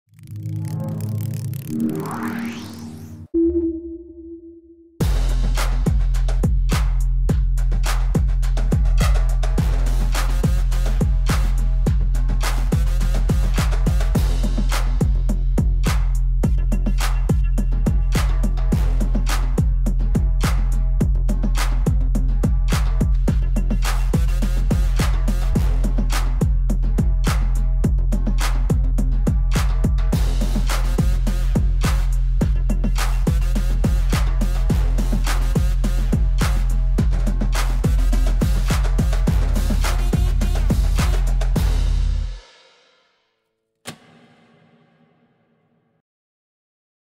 ATOMIC Switch LED SuperLever Panel
Introducing Switch, our state-of-the-art LED SuperLever Panel specially crafted to elevate the visual impact of stages and events. Complete with a custom-printed graphics package, unleash the intergalactic power of dynamic visuals and vibrant colors using backlit LED technology to create an immersive and unforgettable experience for your audience.